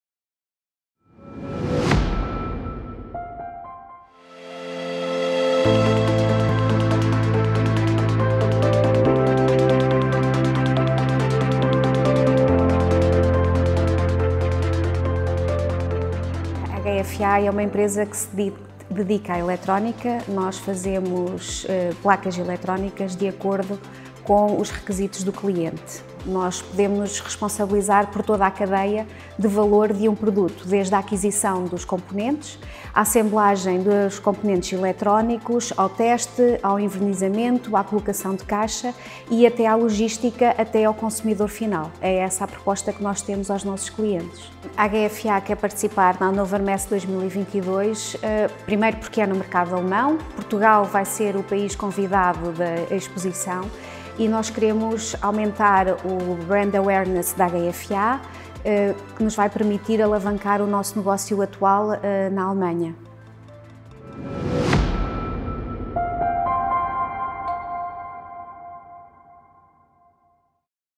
HFA na HANNOVER MESSE 2022
HFA - Henrique, Fernando & Alves, S.A. is a Portuguese SME, specializing in the assembly and testing of electronic and telecommunications equipment.